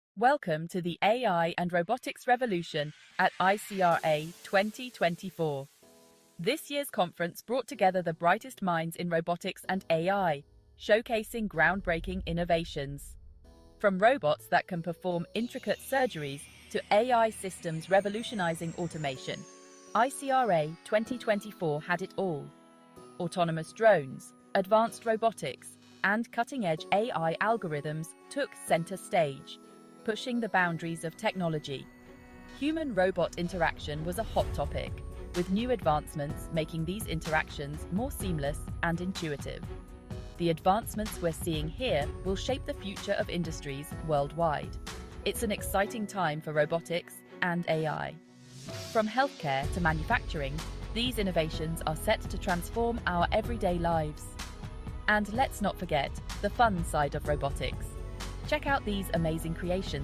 Ai robotics revolution at ICRA 2024
"Discover the latest breakthroughs in AI and robotics from the International Conference on Robotics and Automation (ICRA) 2024! Join us as we explore cutting-edge technologies, from autonomous drones to advanced human-robot interactions. Hear from industry experts about how these innovations will shape the future.